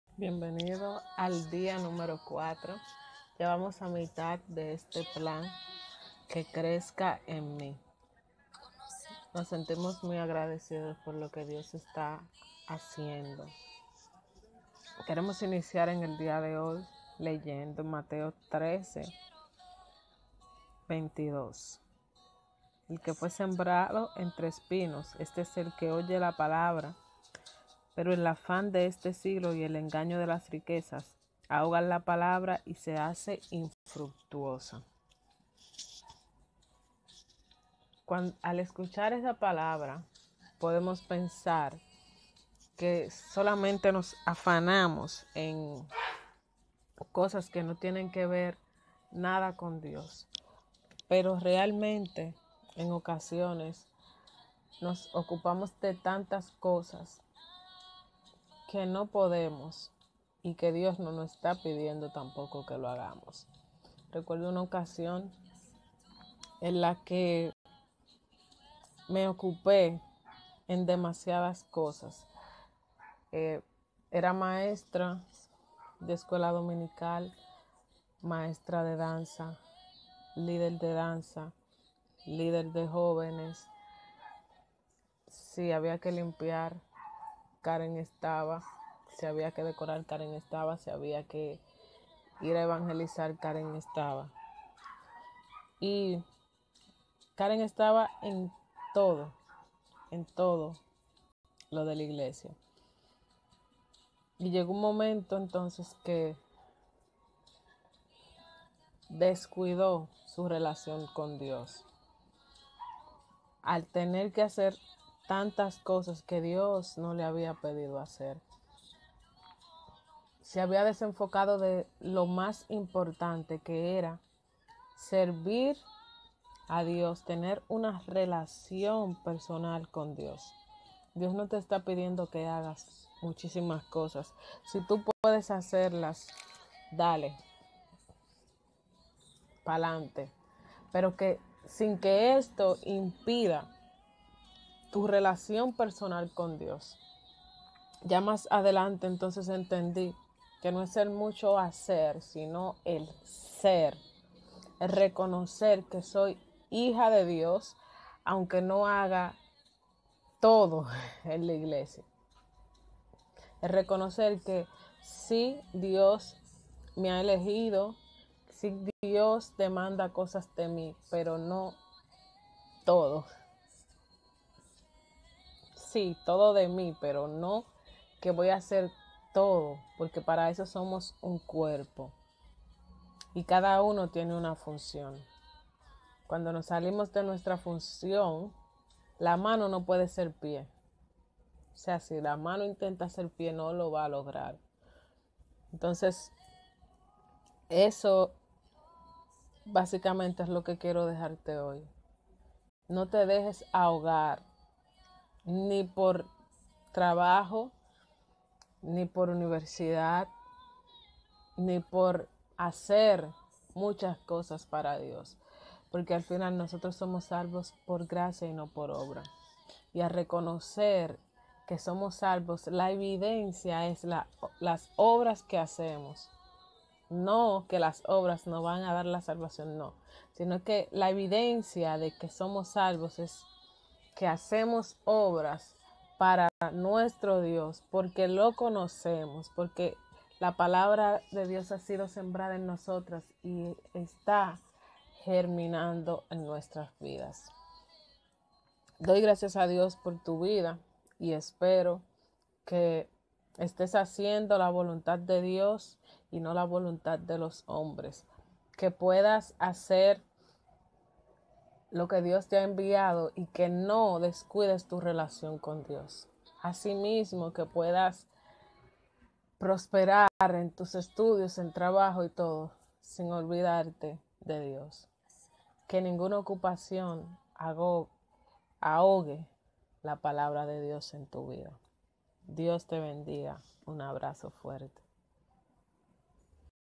Día 4 estudio del plan: Que crezca en mi
Dios te bendiga mucho, en esta ocasión estamos estudiando nuestro primer plan, que crezca en mí, queremos hacerlo junto a ti.

Accede al estudio dando clic en el siguiente enlace; plans/24327/together/53700866/invitation?source=share&token=_zRRnjW3ZWpi3efCGhkOIQ

cuéntanos ¿Cómo te va con el estudio?